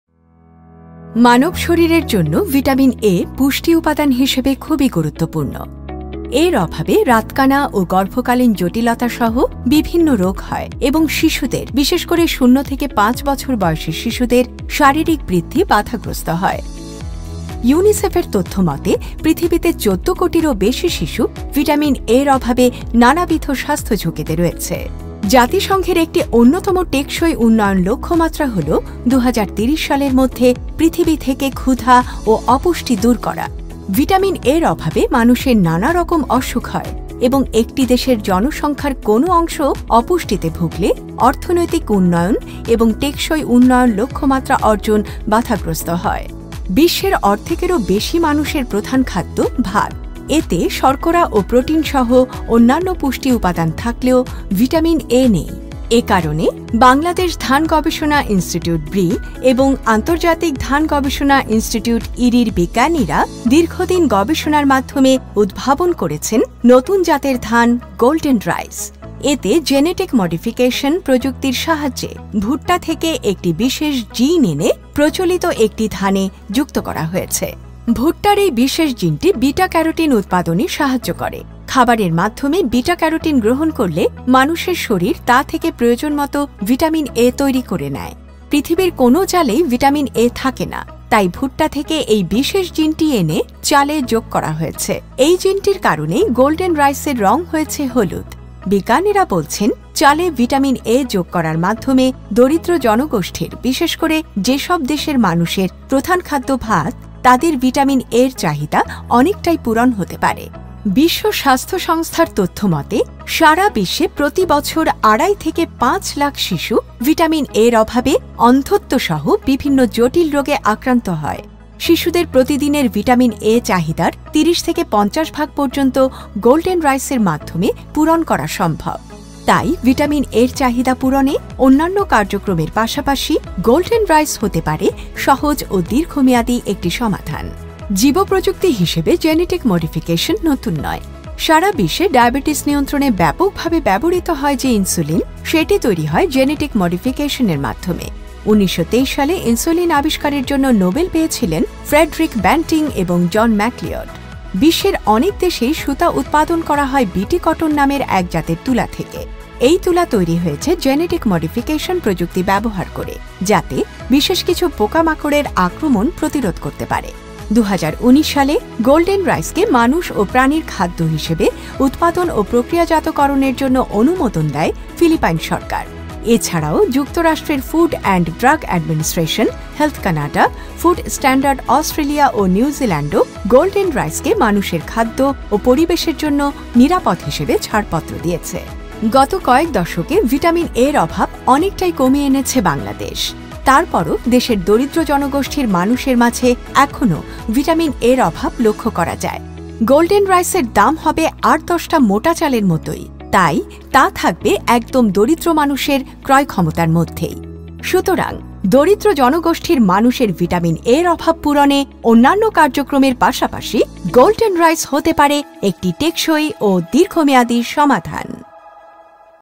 Golden Rice: A complementary nutrition intervention towards ZeroHunger in Bangladesh
Vitamin A deficiency (VAD) can slow down physical growth and cause night blindness among children. It can also lead to pregnancy-time complications for women of reproductive age. As we work towards ZeroHunger by 2030, we need to utilize available nutrition options to ensure that no one is left behind. One of these complementary nutrition interventions is Golden Rice. Watch the video to learn more.

ভিটামিন-এ এর অভাবে রাতকানা ও গর্ভকালীন জটিলতাসহ বিভিন্ন রোগ হয় এবং ০-৫ বছর বয়সী শিশুদের শারীরিক বৃদ্ধি বাধাগ্রস্থ হয়। গত কয়েক দশকে ভিটামিন-এ এর অভাব অনেকটাই কমিয়ে এনেছে বাংলাদেশ। তারপরও, দেশের দরিদ্র্য জনগোষ্ঠীর মানুষের মাঝে এখনো ভিটামিন-এ-এর অভাব লক্ষ্য করা যায়। এ কারণে, দীর্ঘদিনের গবেষণার মাধ্যমে ব্রি এবং ইরির বিজ্ঞানীরা উদ্ভাবন করেছেন নতুন জাতের ভিটামিন-এ সমৃদ্ধ ধান গোল্ডেন রাইস। আরো জানতে ভিডিওটি দেখুন।